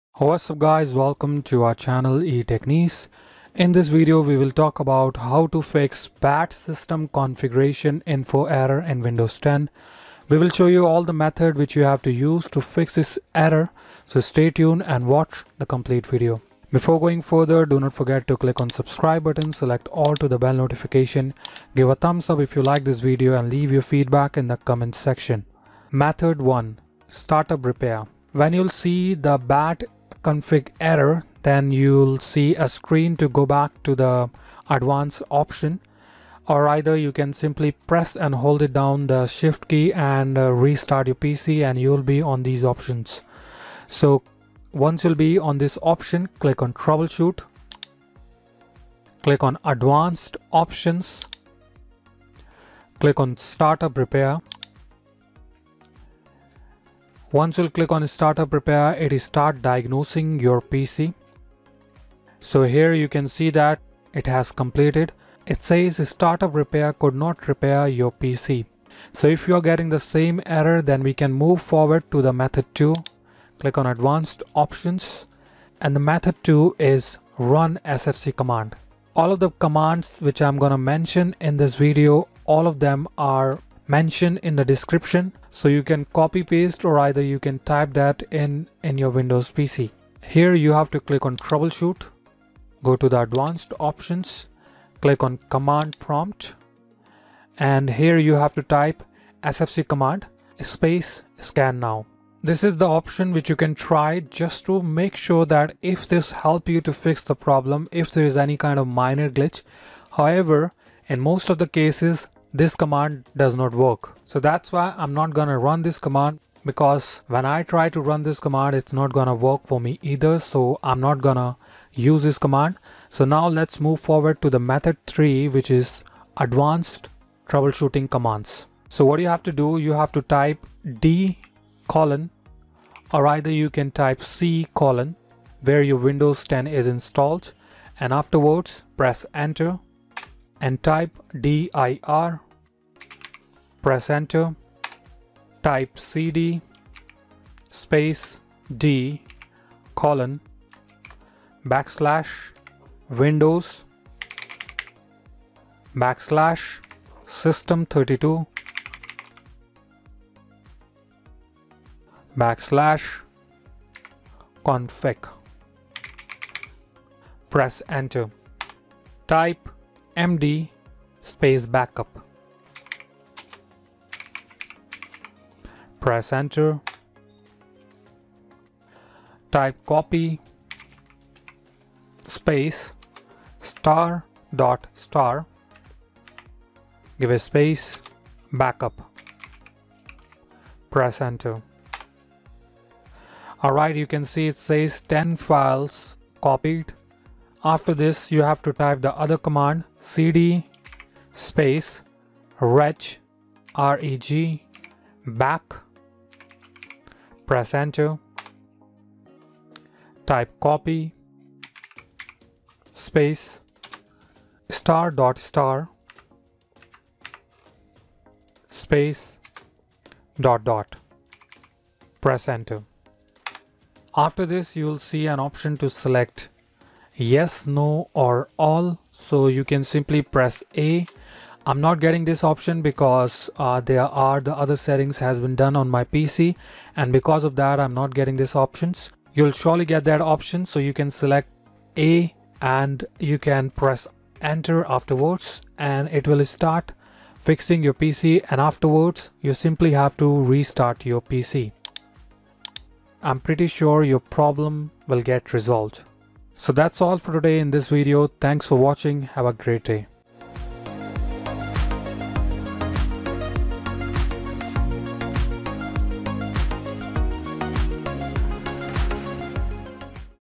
Bad System Config Info Error Fix ( 2025 ) - Step by Step
In this video we have explained how to Fix Bad System Config Info Error and are the methods you have to use to fix BAD_SYSTEM_CONFIG_INFO error.

Commands :

sfc /scannow

d: or c:
dir  Enter
cd d:\windows\system32\config
MD backup
copy *.* backup
CD regback
copy *.* ..
A

When we talk about BAD SYSTEM CONFIG INFO in Windows 10 means the Windows Error which is also know as BSOD error that you have to use the cirtain commands Fix windows Error.

Even if you try Repair Windows 10 it could not be fixed and stuck to reboot loop which means corrupt Windows files or corrupt registry

The Bad System Config Error win 10 is very hard to fix but when there is Bad System Config Error. You can try advaced commands metioned above to fix Bad System Config Error win 10 stuck.